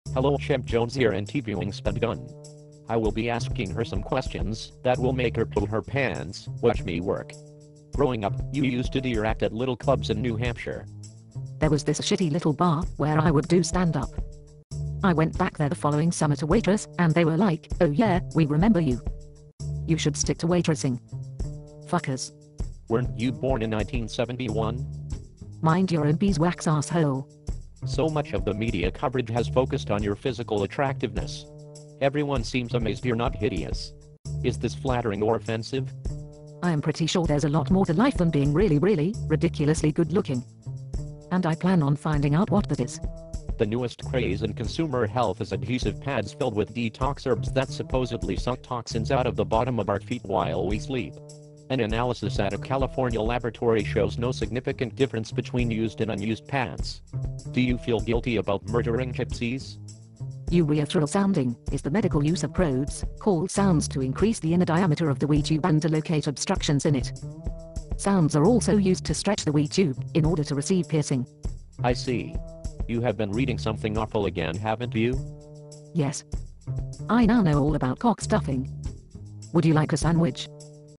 Later On.. The Spudgun Speaks Exclusively about "Things"
After the slow start to the Talk Show.. Spud opens up some under the relentless pressure of Chimp's inteview technique